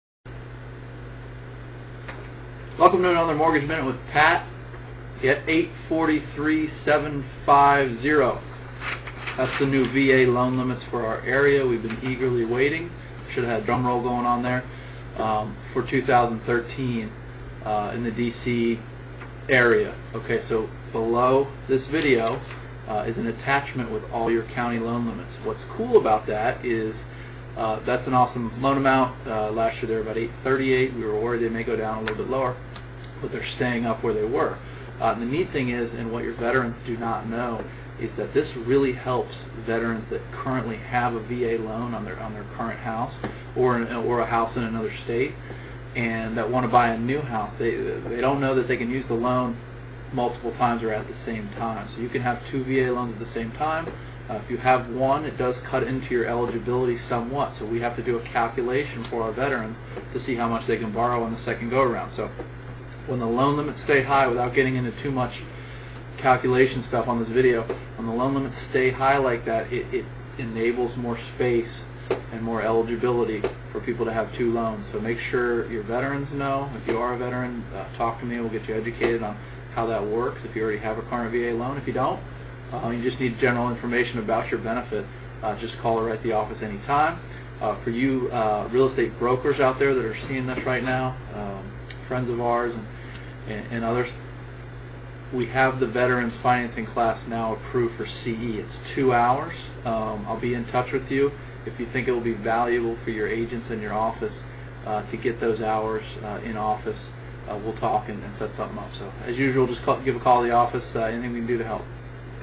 Mortgage Minute | VA Loan Limit
Mortgage Minute | Patrick Cunningham

Patrick Cunningham, Vice President at HST Mortgage with some great information on the new VA Loan limits.

Patrick Cunningham
VP, HST Mortgage